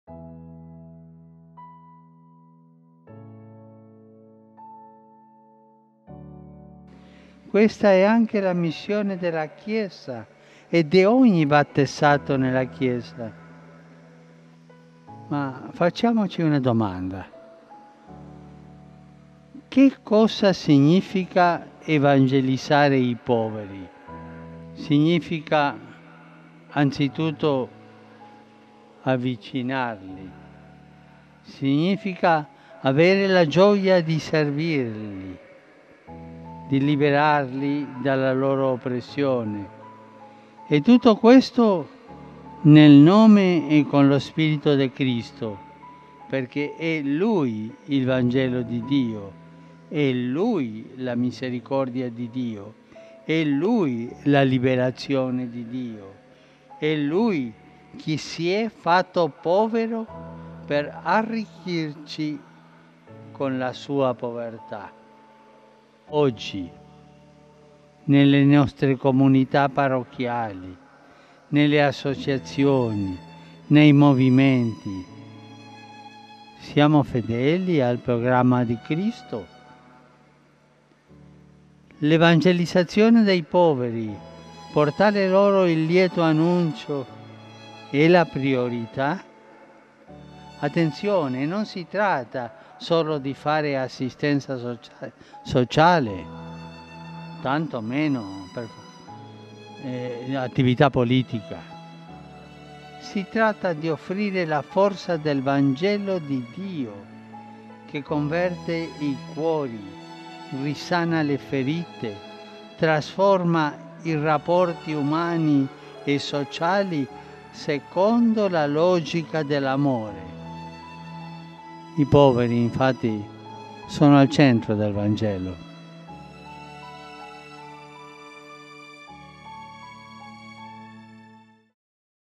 You have a mission to serve the poor, Pope asserts
In his Angelus address in Rome this weekend, Pope Francis said every baptized Christian is called to serve the poor.